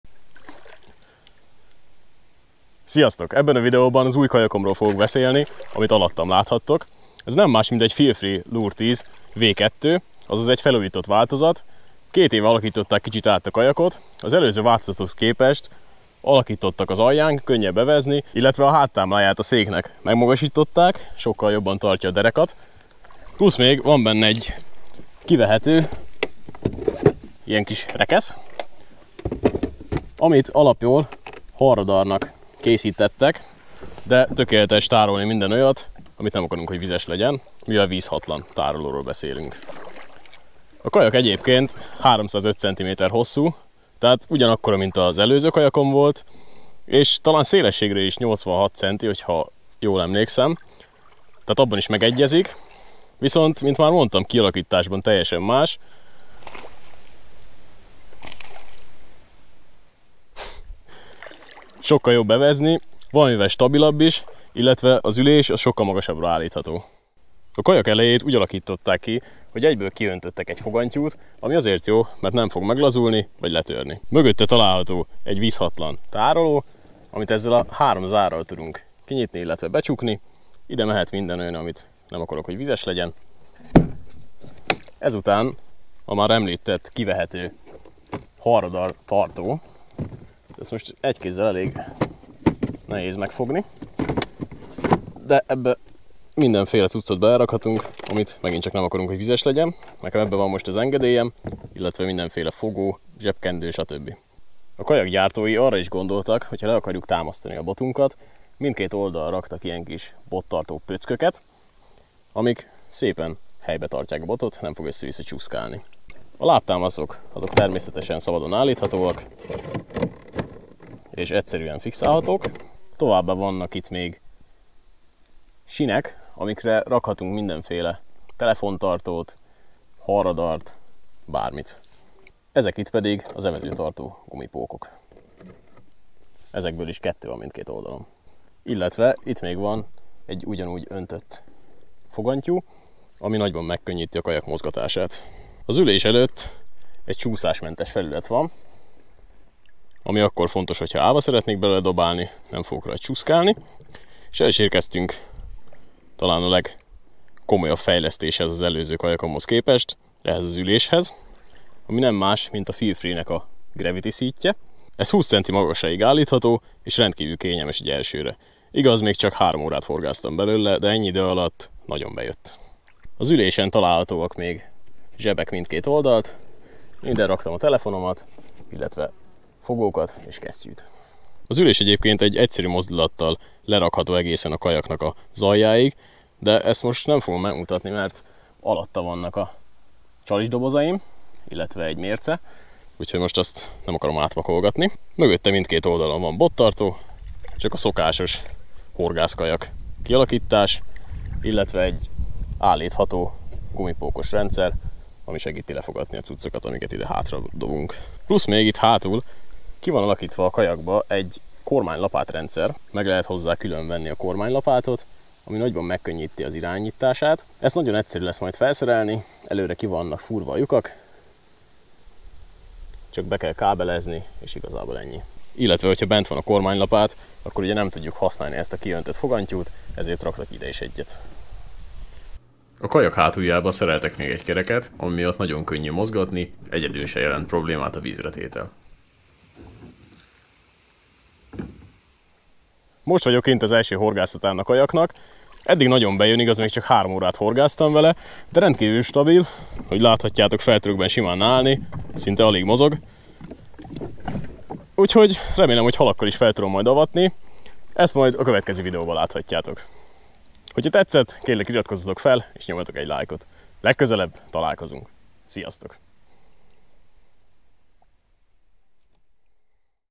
Új kajak - FeelFree Lure 10 V2 bemutató.
Megint lecseréltem a kajakom, de szerintem most pár évre letudtam a kajakvásárlást.Túl vagyok az első horgászatán az új szerzeménynek, és eszméletlenül tetszik, nagyon kényelmes,stabil, eddig úgy néz ki minden igényemet ki fogja elégíteni.
Nemsokára jelentkezem a kajak első horgászatával is.

Ha támogatni szeretnéd a csatorna fejlődését,azt a "Köszönet" lehetőségnél a videó alatt megteheted.